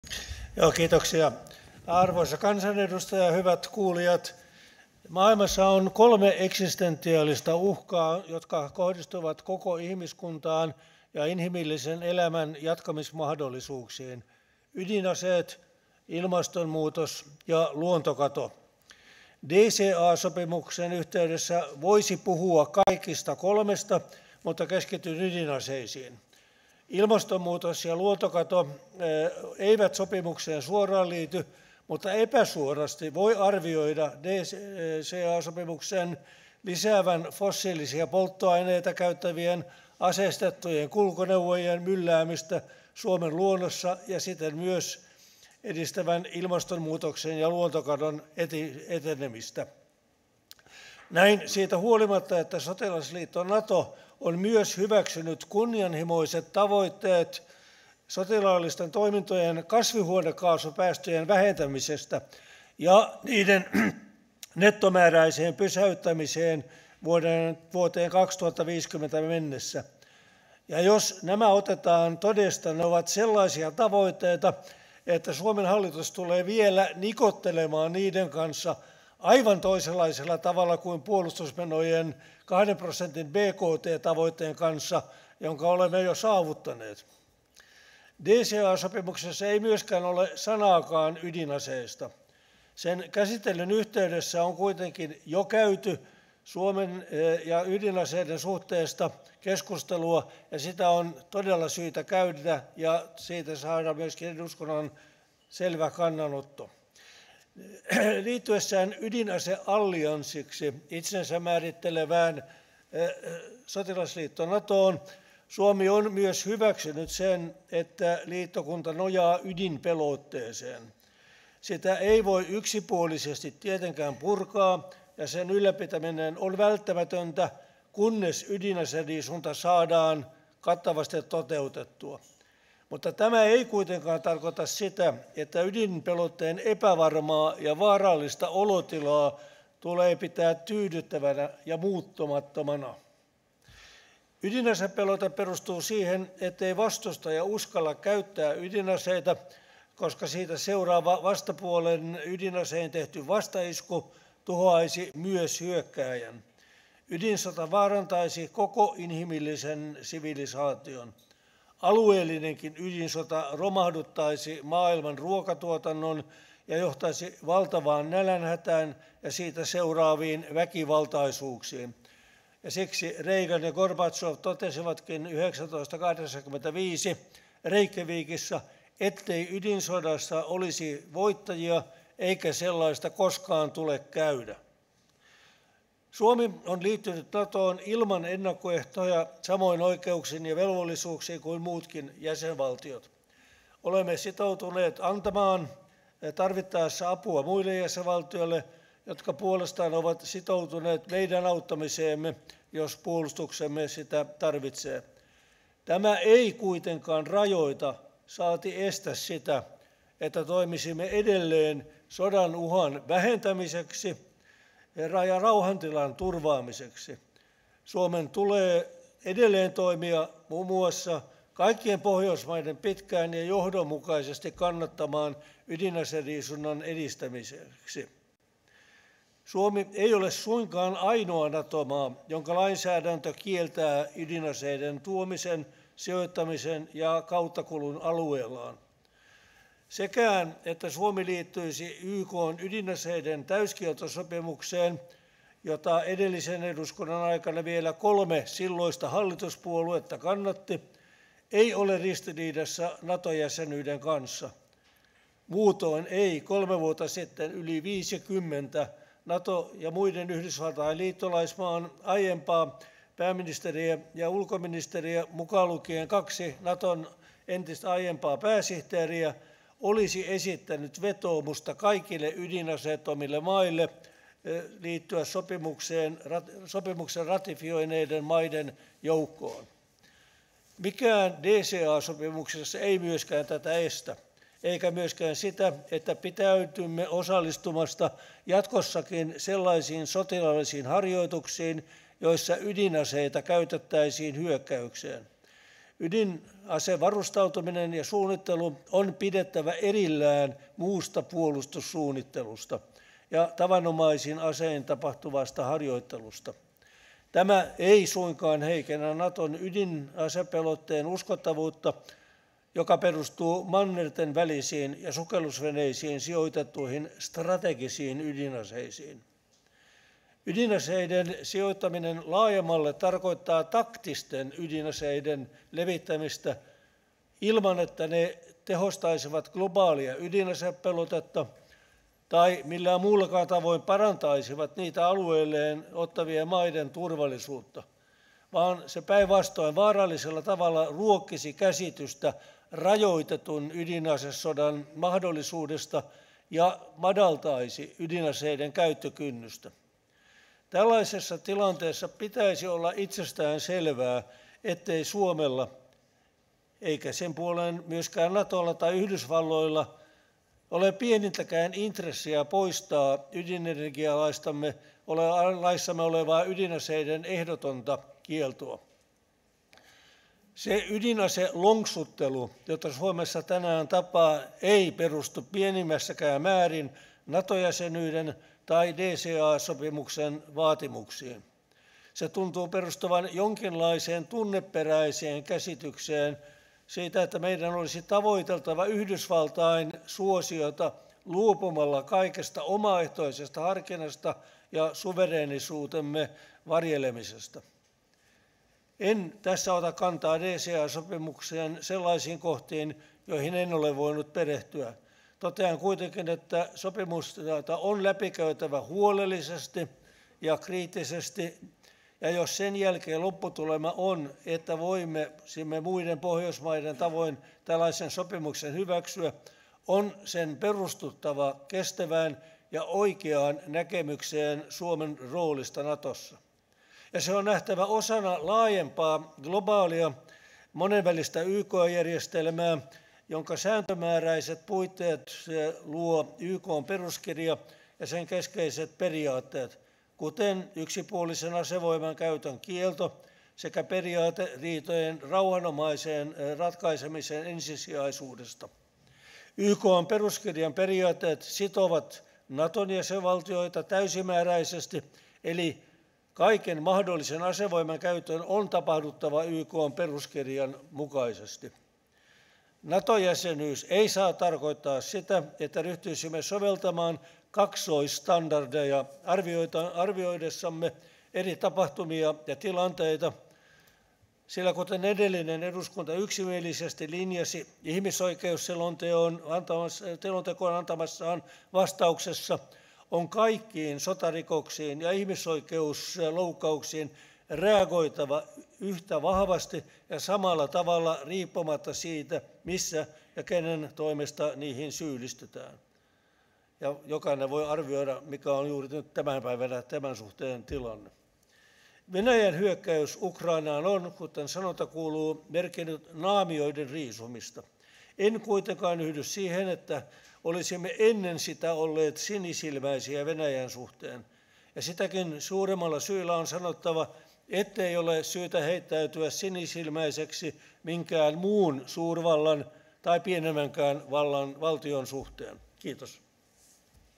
kuuleminen Suomen ja Yhdysvaltojen puolutussopimuksesta - ydinaseet
Erkki Tuomioja käsittelee ydinaseita, ilmastomuutosta ja luontokatoa ihmiskunnan uhkina.